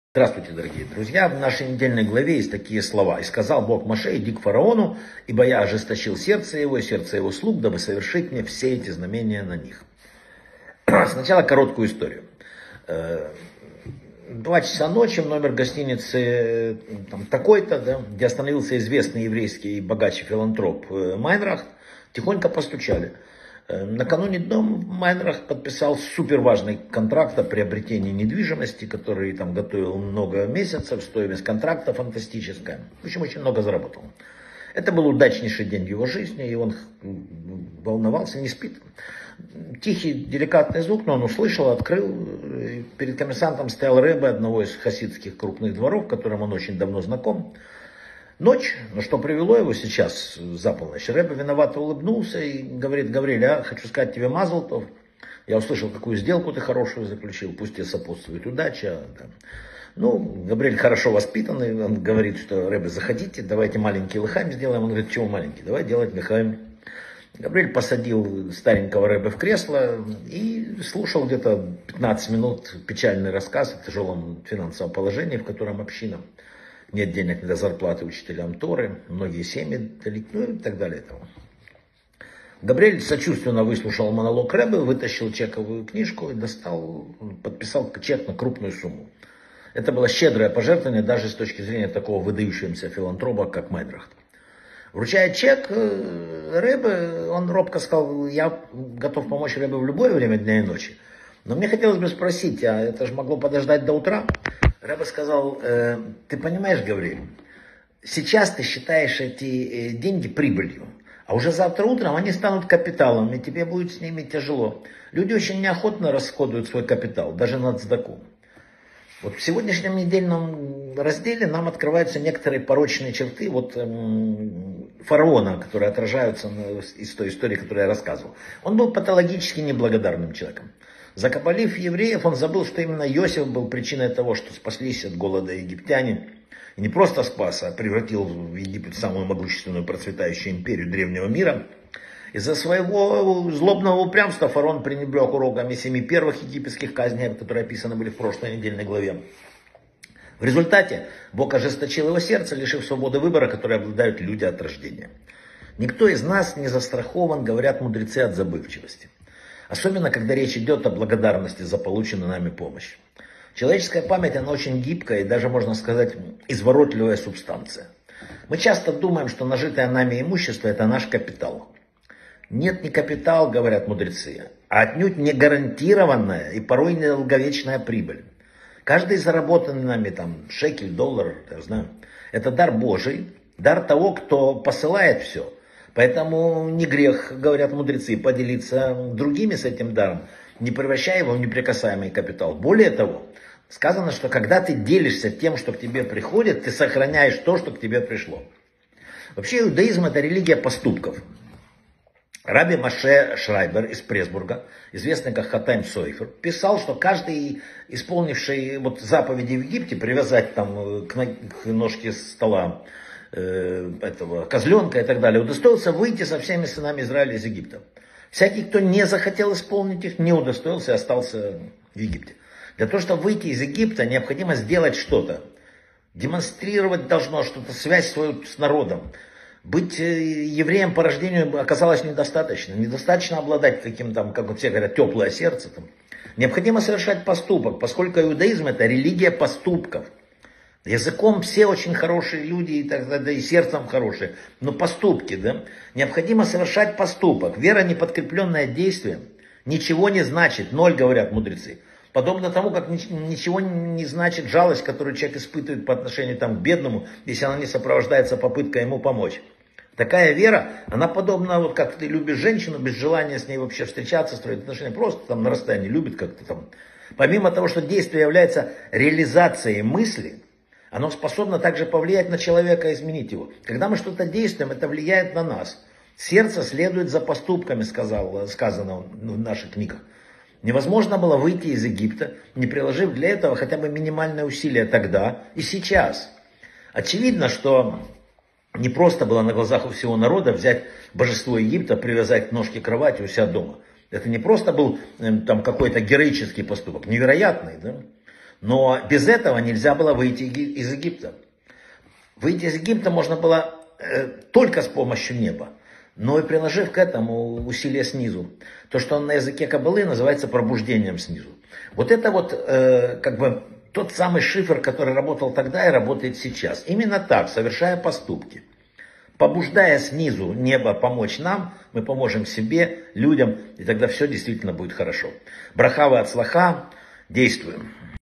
📘 Человек должен делиться, чтобы сохранить. Недельная глава Бо - Урок 3 | Вадим Рабинович
Почему человек начинает испытывать неблагодарность, когда увязает в плену материальности? Путь фараона.
История о богаче по фамилии Майнрах, заключившем невероятно выгодную сделку и раввине общины, пришедшем к нему в два часа ночи.
Память - гибкая и изворотливая субстанция.
Почему все заработанное, что мы привыкли считать своим капиталом, на самом деле непостоянно и как человек должен правильно относиться к тем ценностям, которые он получает?
Иудаизм - это религия поступков. Что писал Хатам Софер о роли исполнения заповеди привязать ягненка к ножке кровати перед Исходом из Египта?
Вера должна подкрепляться действием. Действие - это реализация мысли и то, что влияет на нас самих.
Помощь Небес и важность наших усилий - то, что необходимо для осуществления цели.